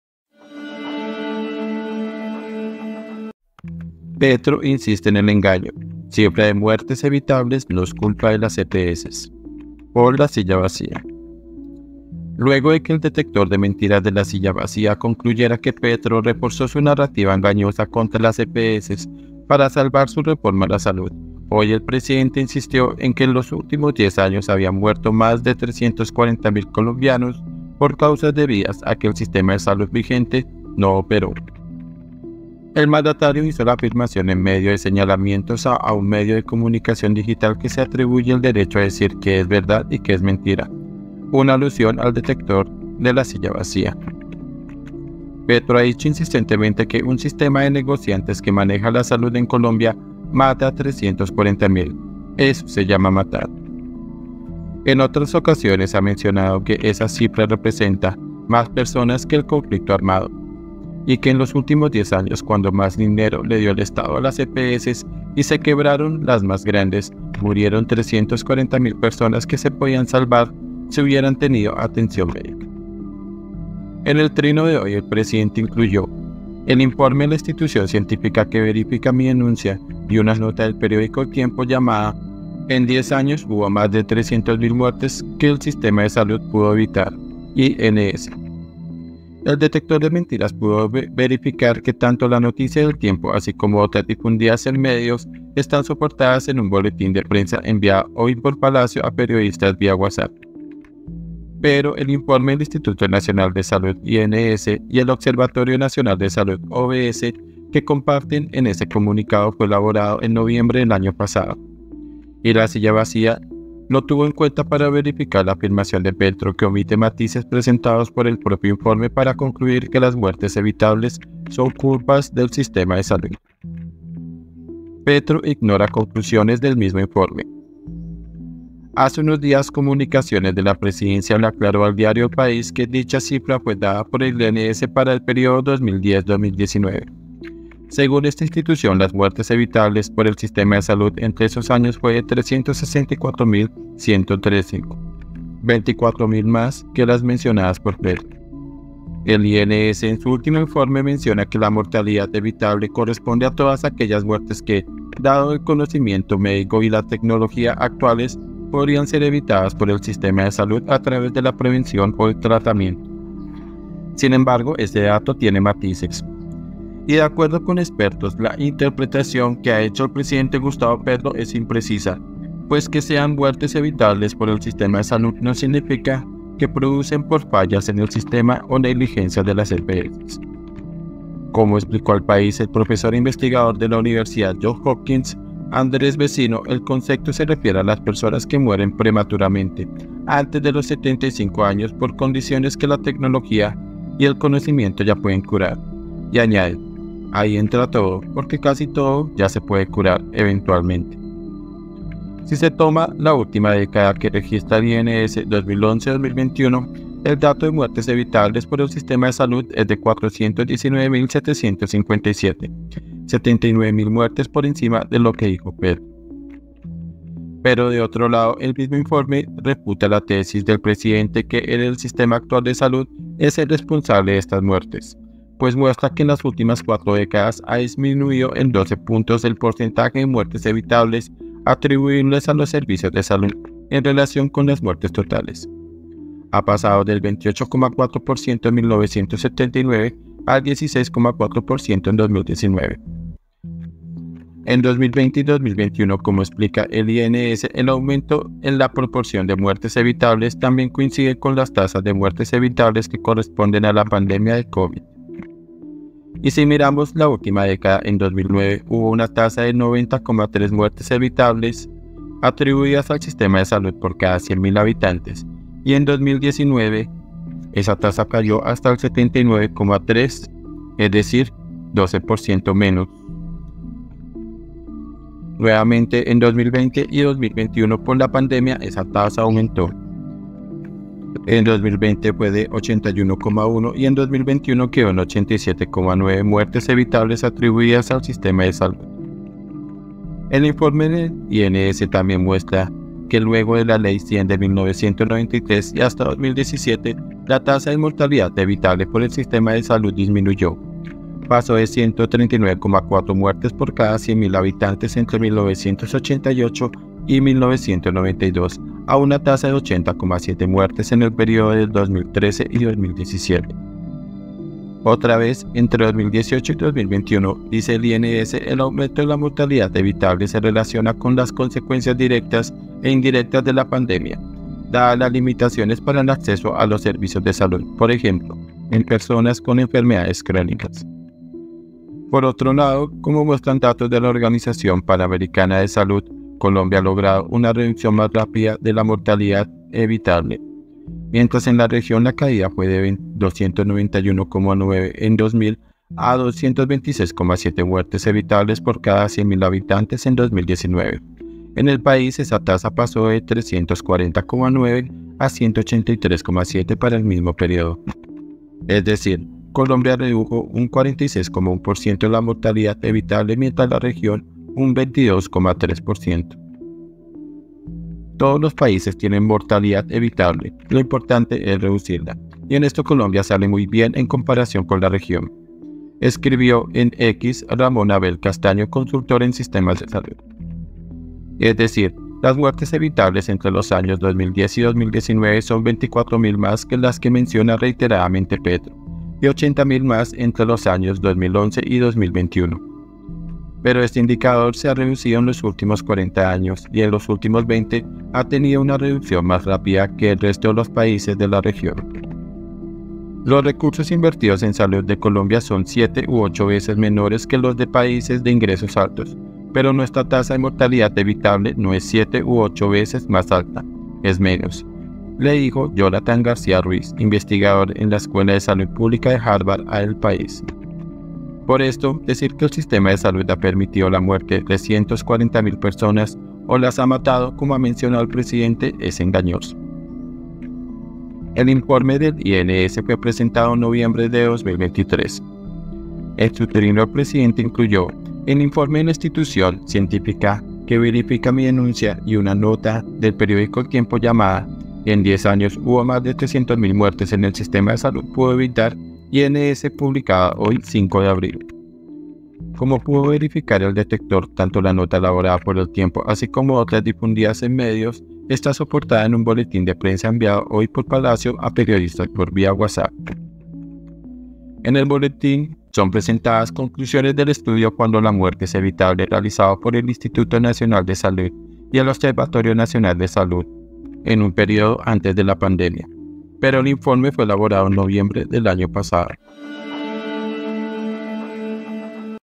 PETRO INSISTE EN EL ENGAÑO ⚫ CIFRA DE MUERTES EVITABLES NO ES CULPA DE EPS
Luego de que el Detector de Mentiras de La Silla Vacía concluyera que Petro reforzó su narrativa engañosa contra las EPS para salvar su reforma a la salud, hoy el presidente insistió en que en “los últimos 10 años habían muerto más de 340.000 colombianos por causas debidas a que el sistema de salud vigente no operó”.